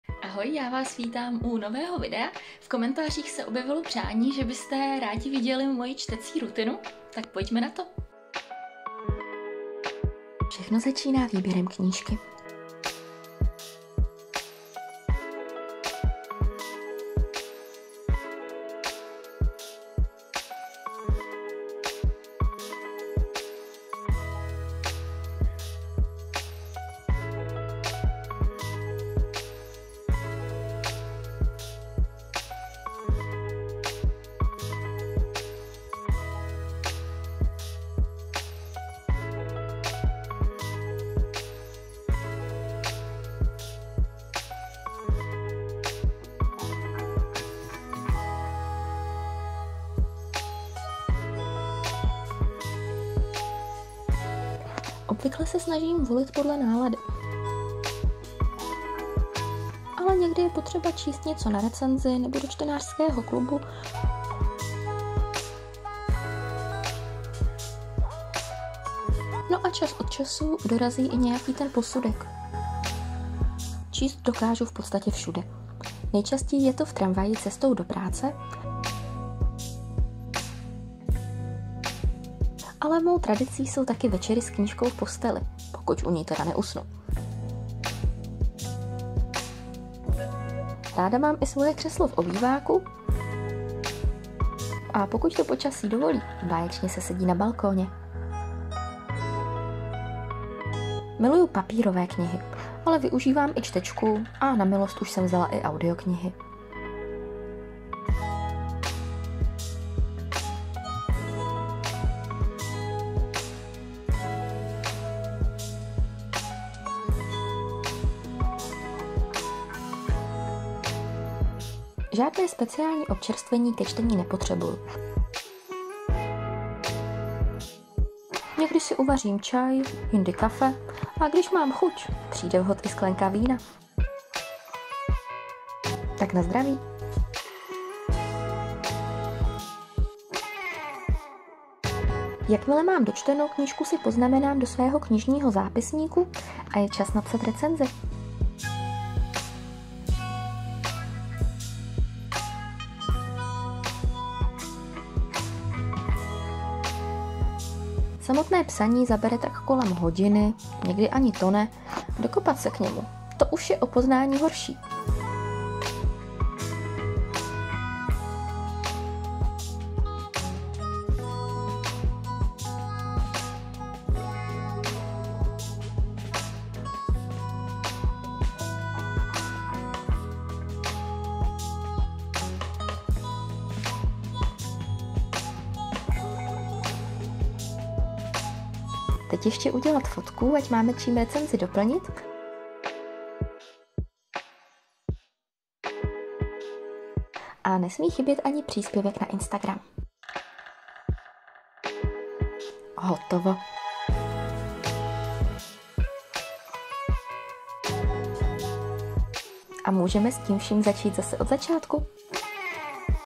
Moje čtecí rutina
Ahoj všichni,
chtěli jste vidět, jak vypadá moje čtecí rutina, tak tady to máte. :)
A příští týden už se můžete zase těšit u klasického povídacího videa. :)

Máte nějakou připomínku k mojí tvorbě, chcete se na něco zeptat nebo mi prostě jen chcete napsat?

Budu ráda, když mě navštívíte i na mých dalších sítích. :)